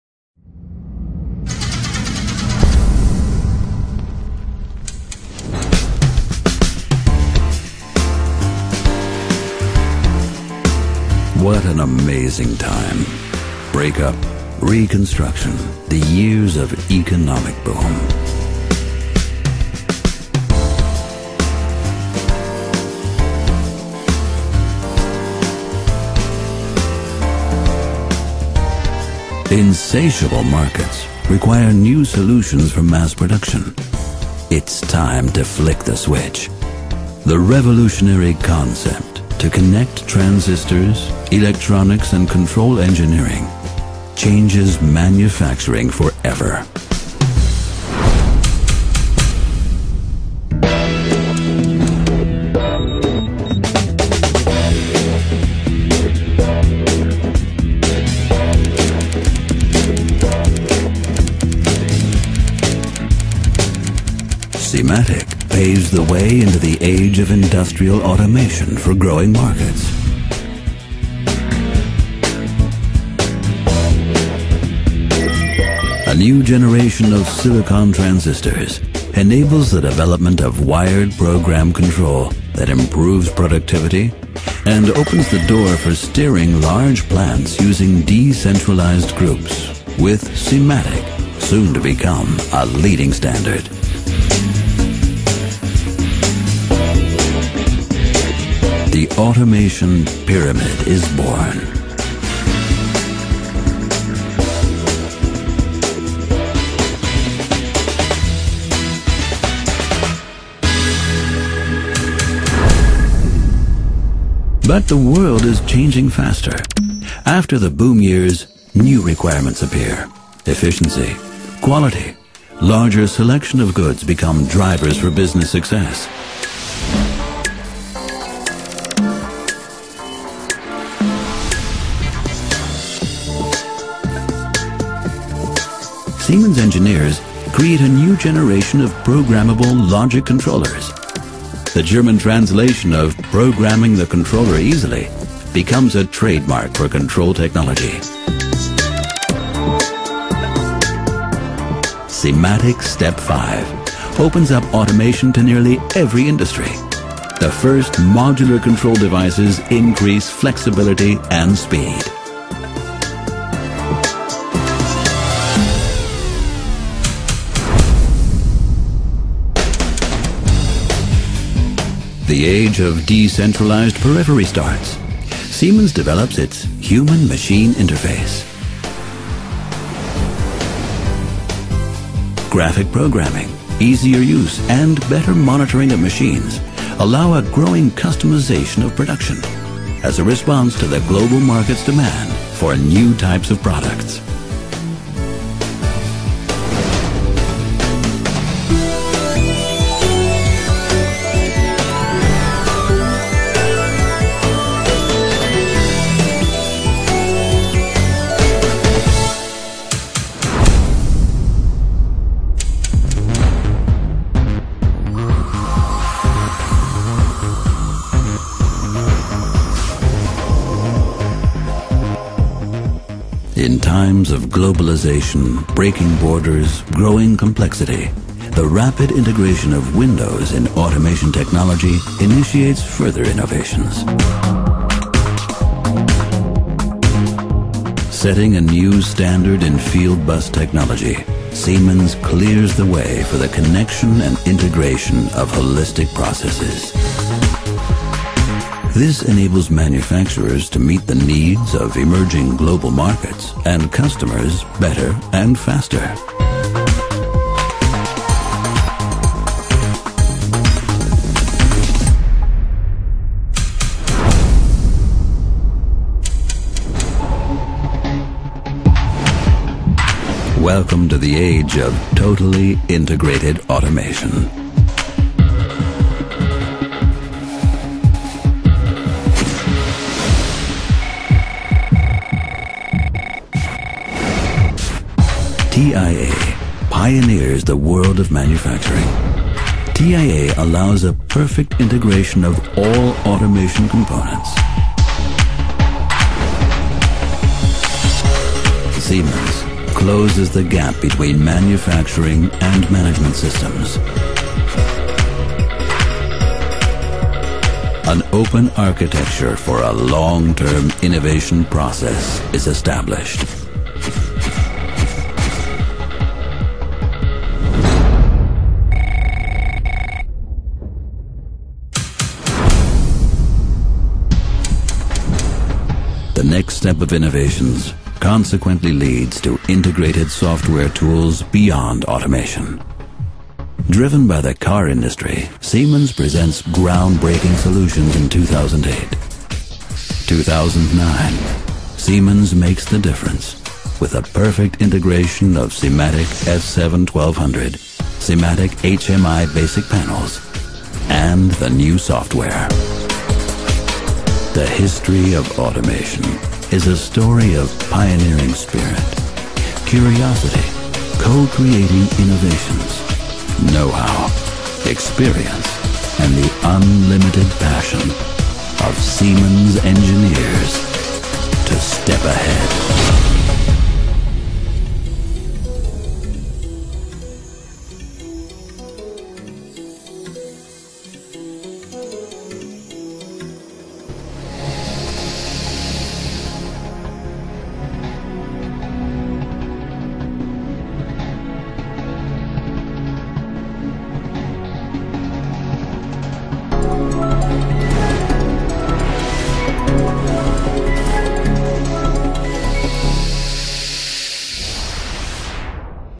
SIMATIC - Historia
Desde Abril de 1958, cuando Siemens Schuckert AG registró en las oficinas de patentes alemanas la marca SIMATIC, sintetizando con ella los nombres de "Siemens" y "Automatización" en un sólo concepto, lo que se gestó como un sistema de control automático revolucionario, se terminaría convirtiendo en el más innovador de los controladores PLC, ofreciendo soluciones que revolucionarían definitivamente el concepto de automatización industrial.